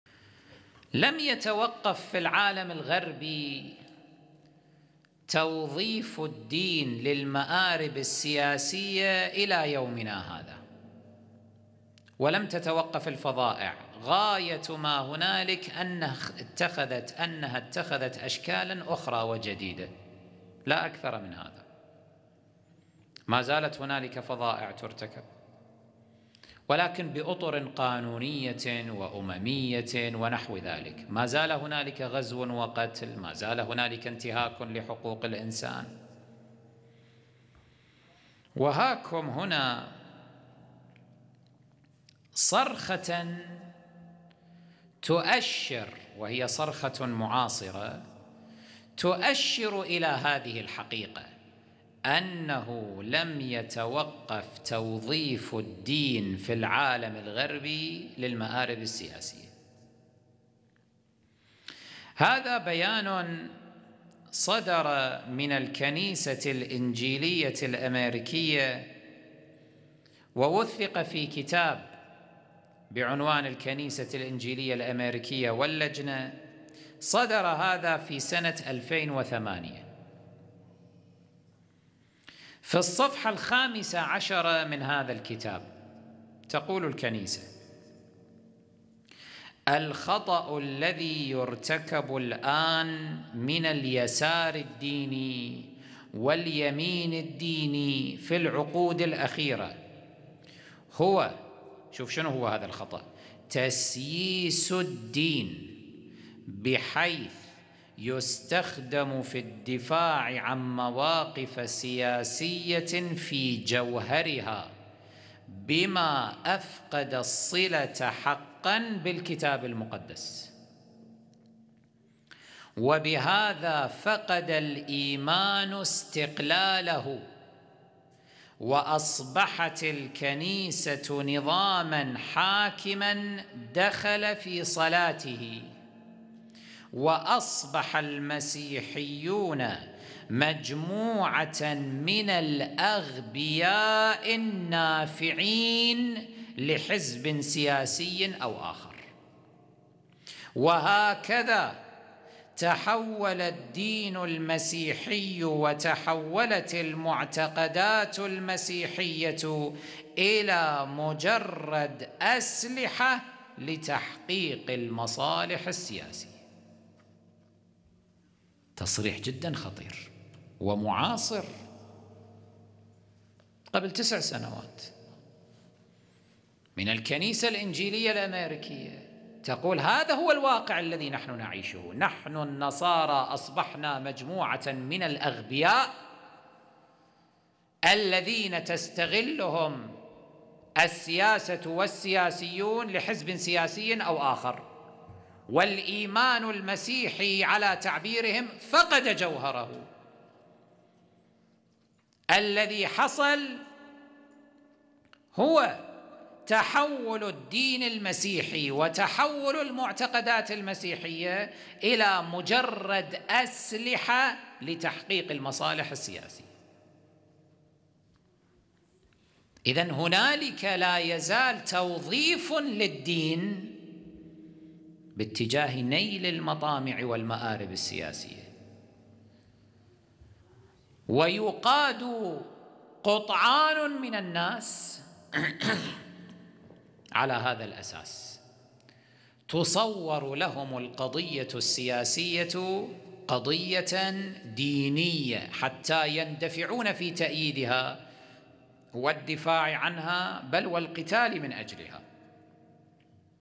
ولا يزال الساسة الغربيين يستخدمون النصرانية لمآربهم
مقطع من البث المباشر في الليلة التاسعة من مجلس الليالي الحسينية لسنة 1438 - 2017 مع الشيخ ياسر الحبيب

مسجد المحسن الشهيد (عليه السلام) ــ لندن